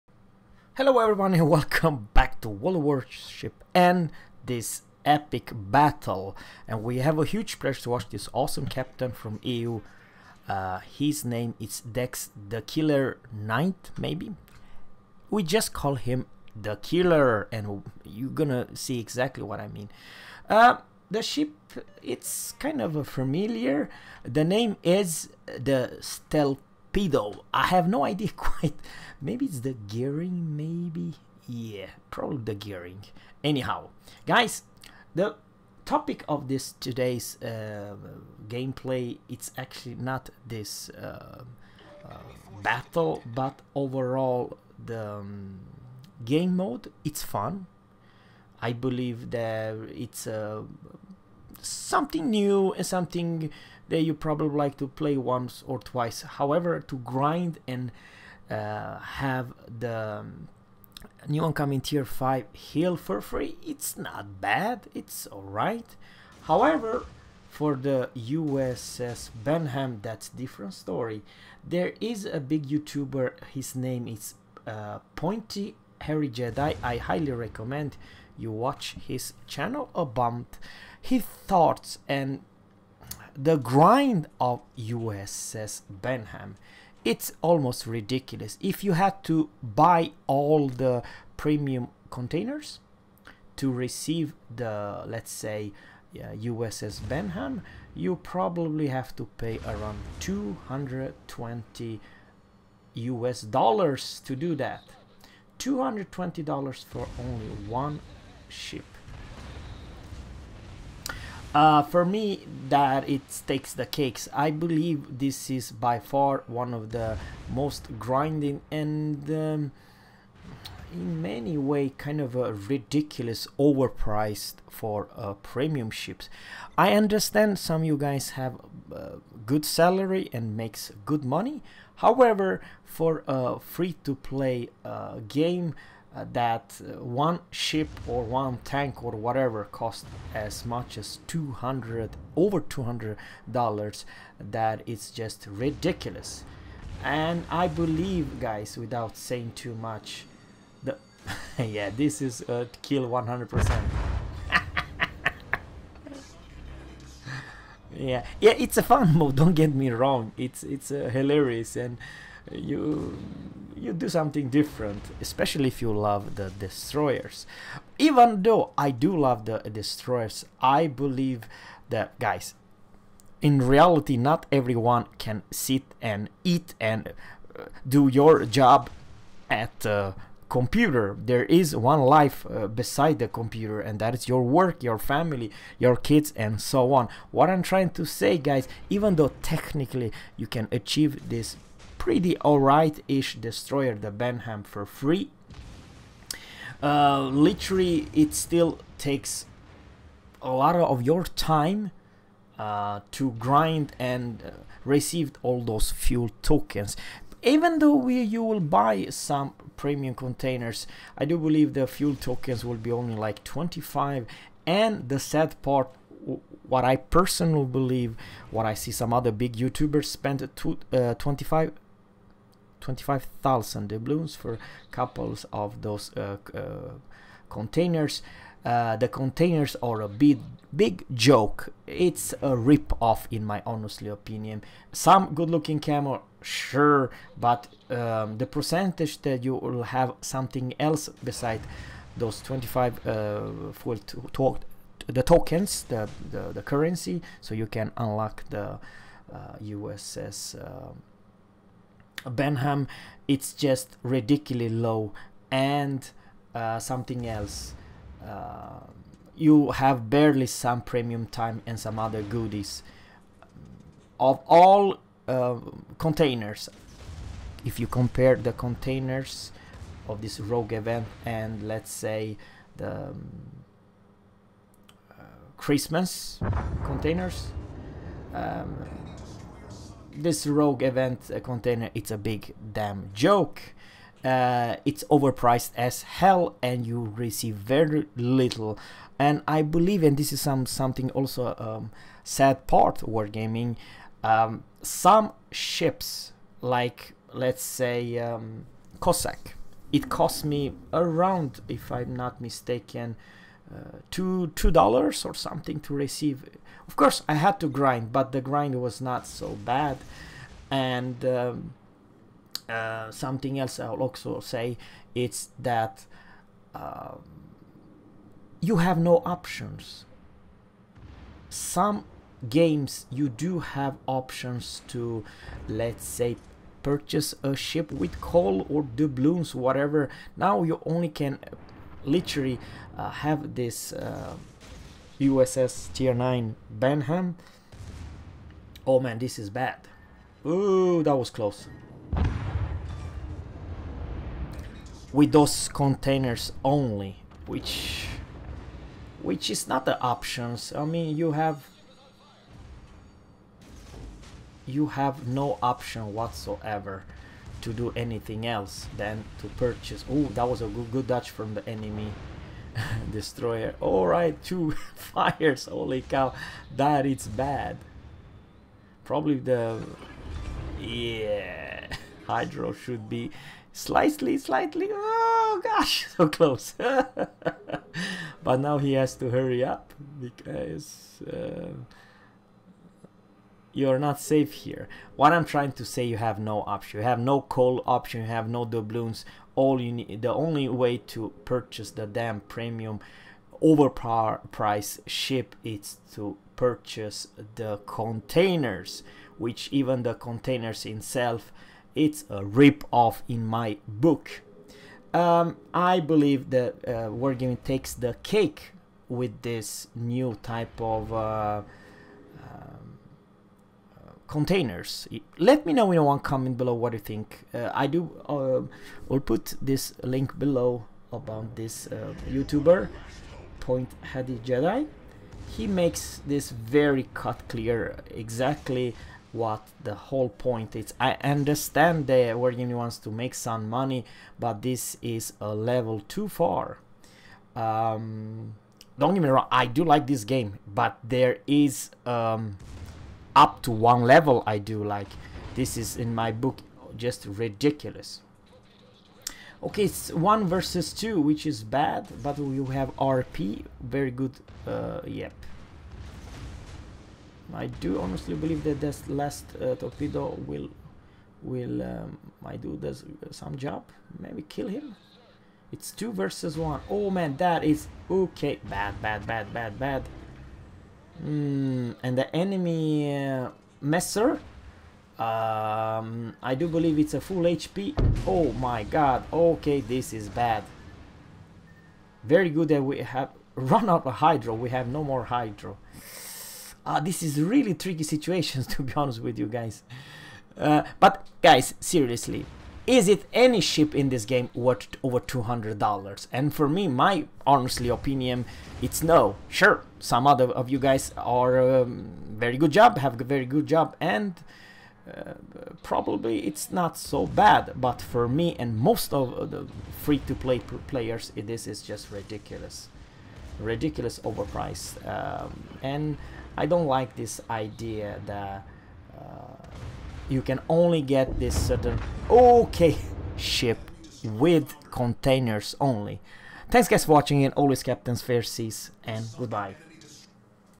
USS BENHAM- Drastically Overpriced !!
If you do not have time to get the Fuel tokens
Are you willing to pay over 220 US Dollars to get the Benham ??
In my book there is NO ship in the game worth 220 US Dollars ..
Let me know in one comment bellow ..